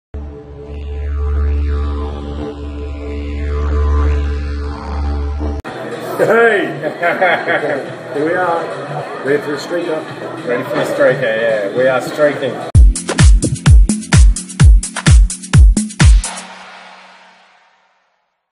Beach Bum Surfaris - Adventure Gold Coast - 6:37pm Friday 2nd of December 2011
Gold Coast Sevens - Fever Pitch. This is definitely where rugby meets party and is a must to include on your calendar each year.